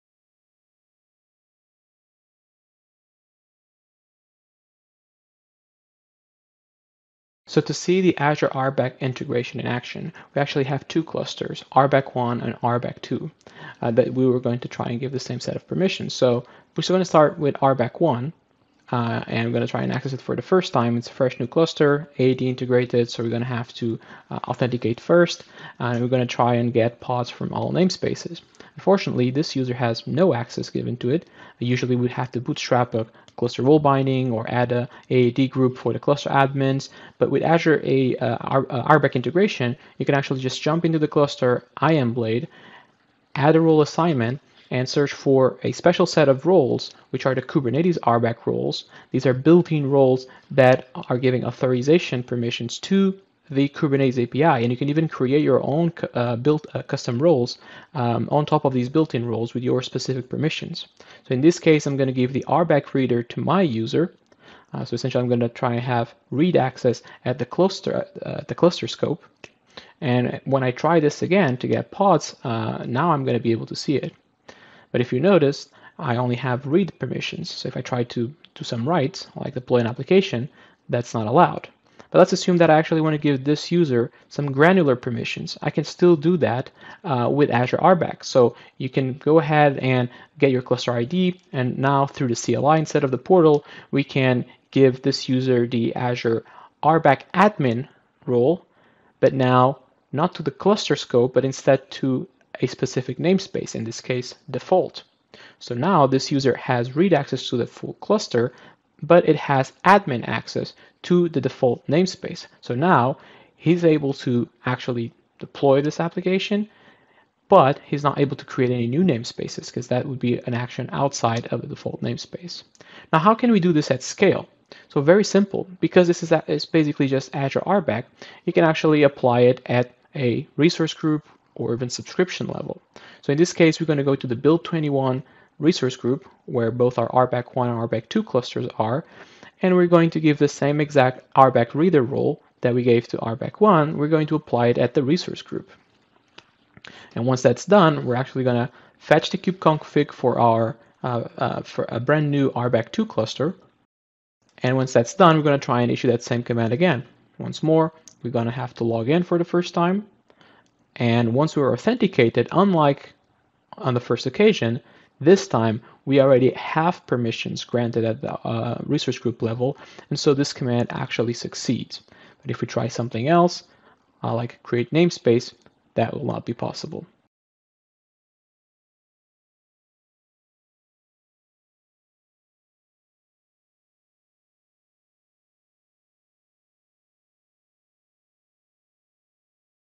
Azure Role-based Action Control (RBAC) for Azure Kubernetes Services | KubeCon NA 2021
This video demonstrates Azure Role-based Action Control (RBAC) integration with Microsoft Azure Kubernetes Services (AKS) and how you can use it to manage access to AKS clusters.